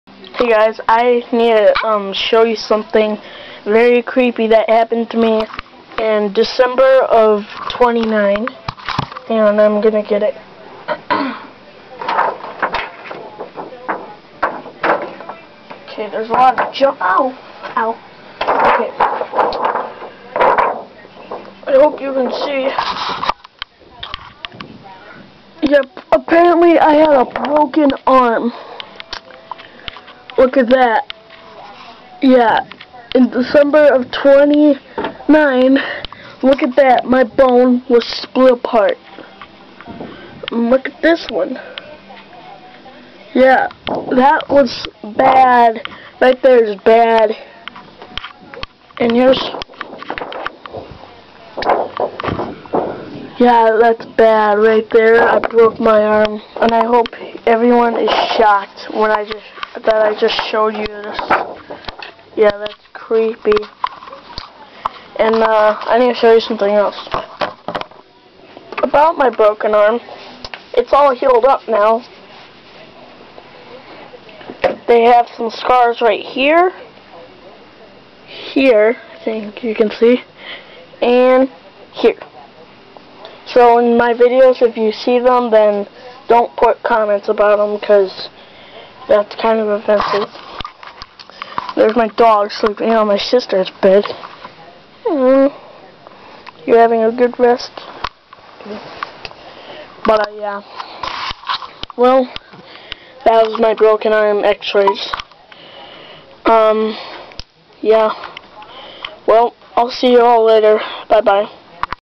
My X-Rays
This is My X-Ray of my Broken Arm. The X-Ray was made in December 2009. it's pretty intresting AND scary!